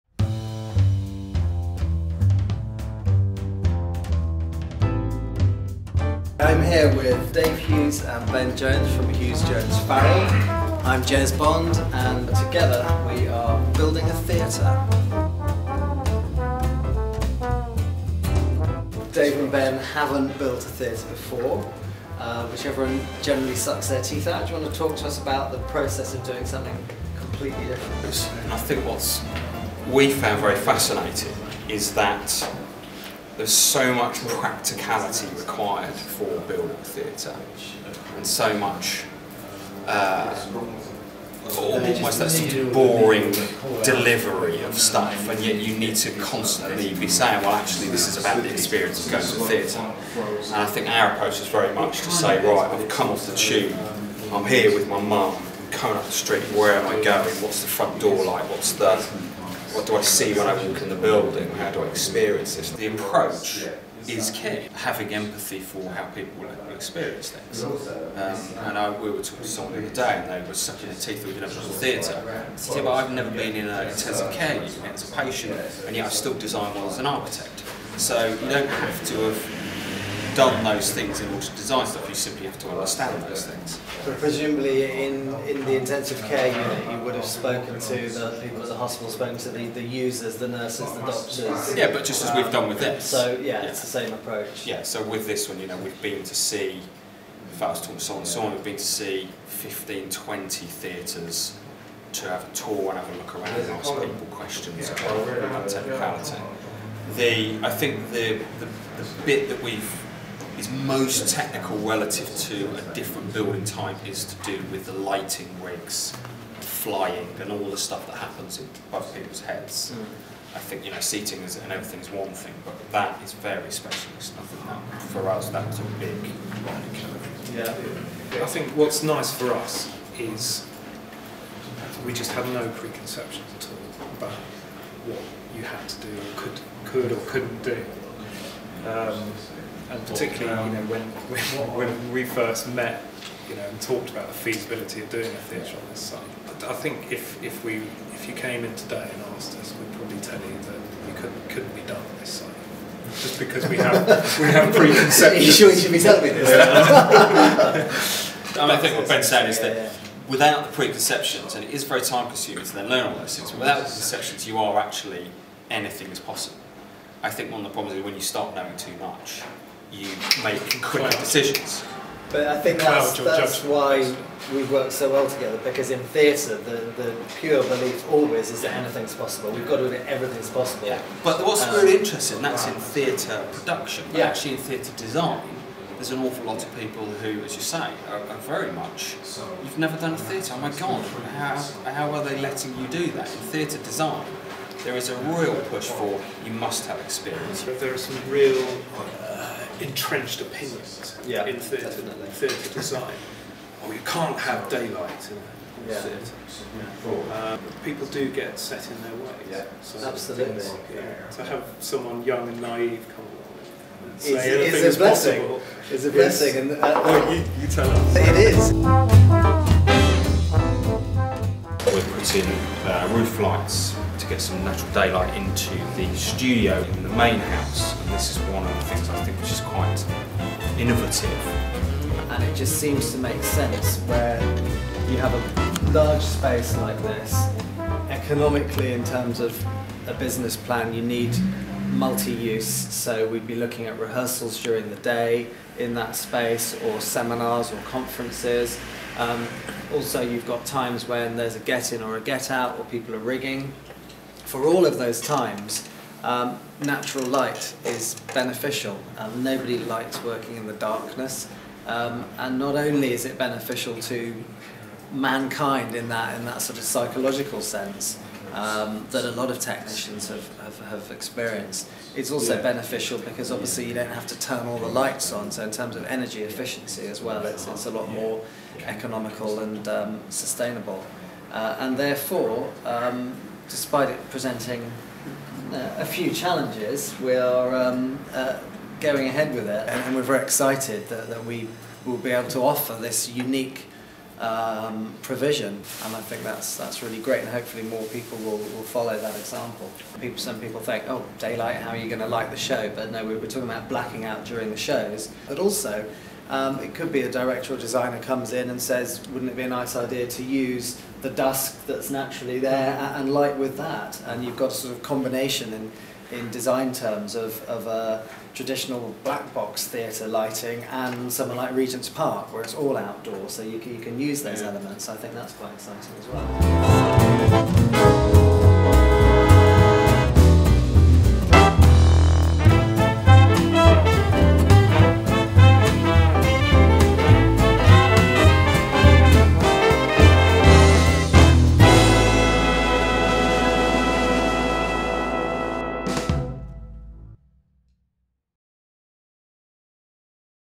HOW TO BUILD A THEATRE - EPISODE 2.mov
Due to open in 2012, The Park Theatre will be a 200 seat main house and 90 seat studio theatre. This is the second installment of the video record of this gigantic project...